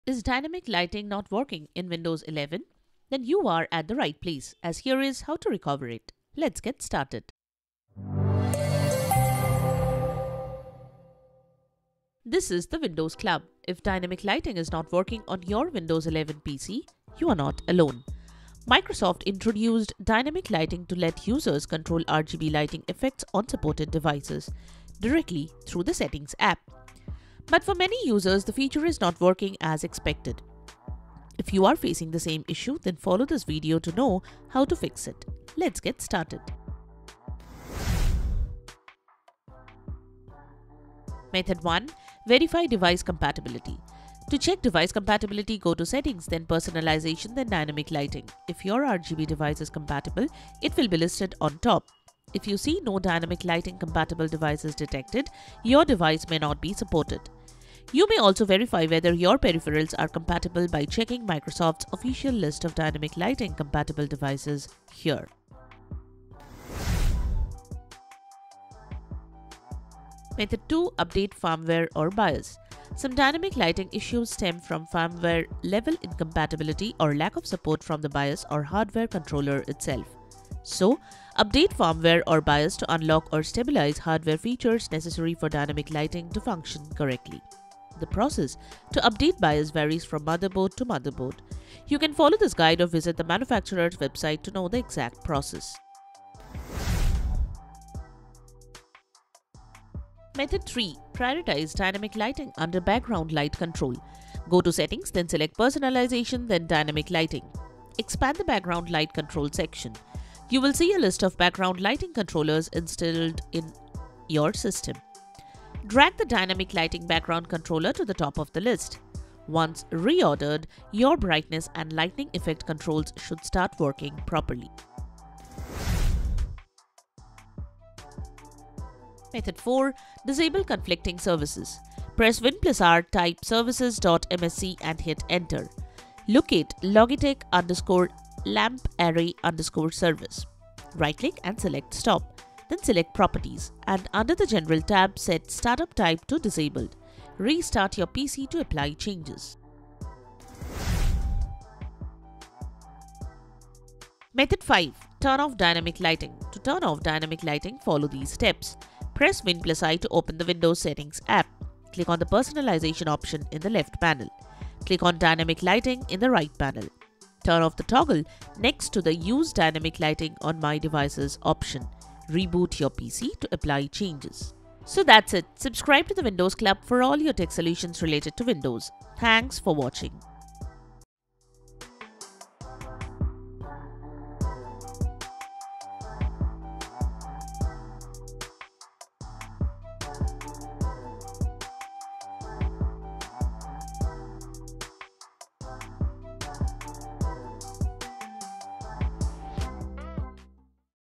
Dynamic lighting not working in Windows 11
If Dynamic Lighting is not working on your Windows 11 PC, you’re not alone. Microsoft introduced Dynamic Lighting, allowing users to control RGB lighting effects on supported devices directly through the Settings app.

But for some users, the feature is not working as expected. If you are facing the same issue, then follow this video to know how to fix it.

00:00 Intro
00:39 Verify Device Compatibility
01:08 Update Firmware/BIOS
01:38 Prioritize Dynamic Lighting under Background light control
02:07 Disable Conflicting Services
02:32 Turn off Dynamic Lighting

Run : services.msc